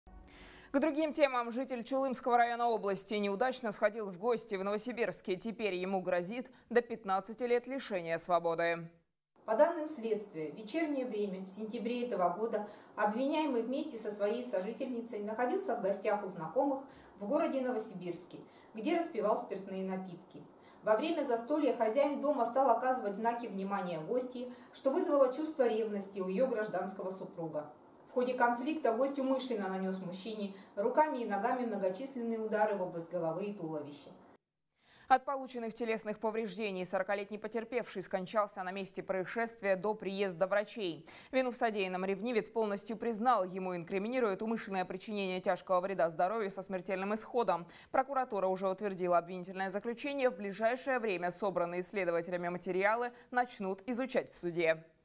Поход в гости закончился уголовным делом для жителя Новосибирской области
Житель Чулымского района области неудачно сходил в гости в Новосибирске. Теперь ему грозит до 15 лет лишения свободы. Во время застолья хозяин дома стал оказывать знаки внимания гостье. Её гражданский муж приревновал и забил искусителя до смерти.